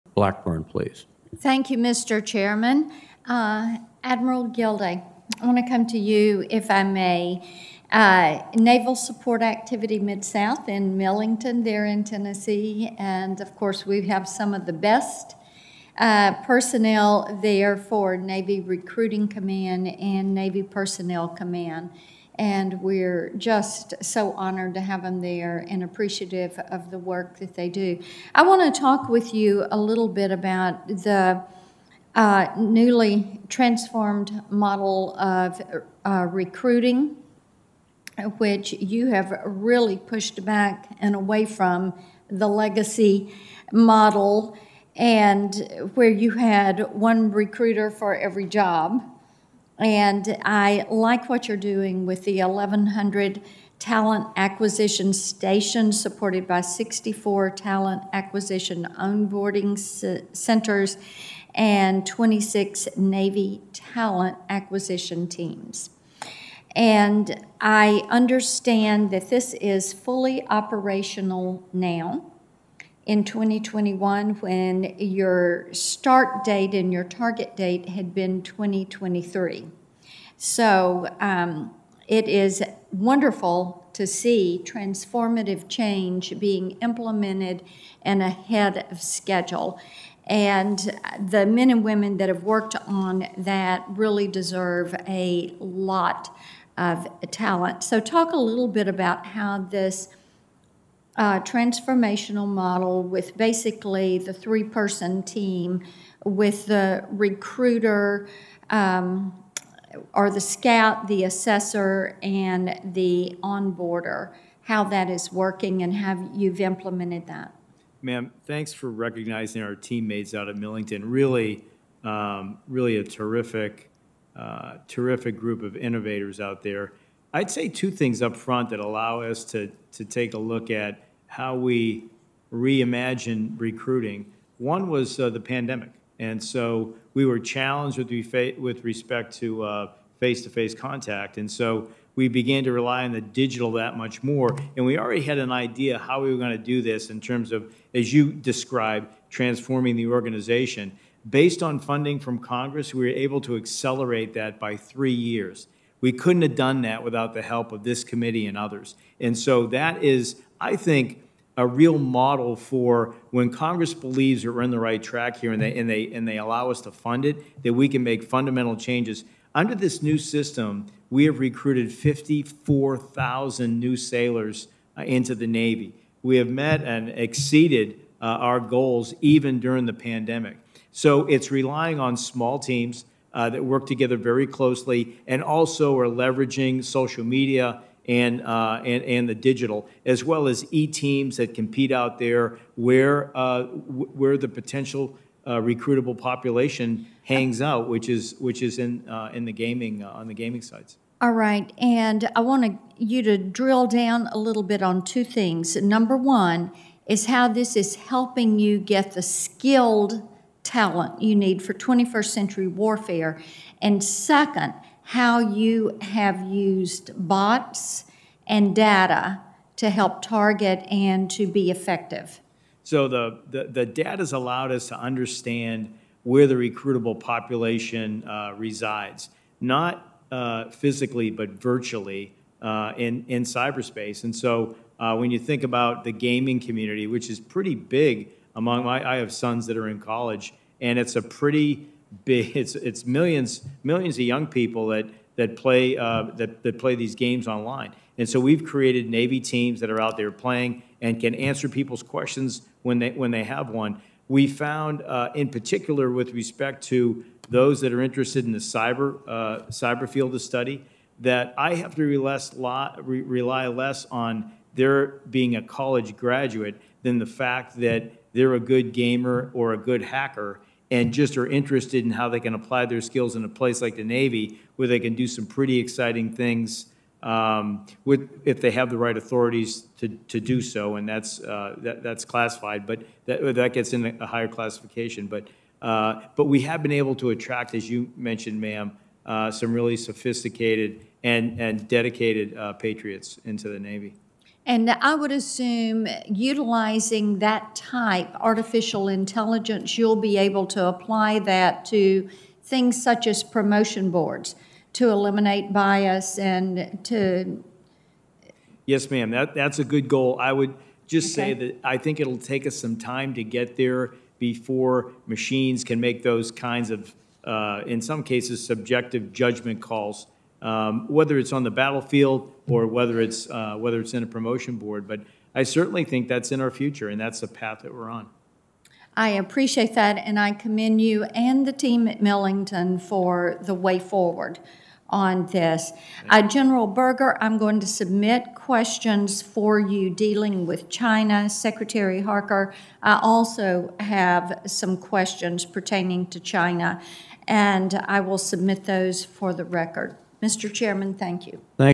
"How Are We Getting Skilled Talent For 21st Century Warfare?" Says Blackburn
Senator Marsha Blackburn joins the Armed Services Hearing to give remarks.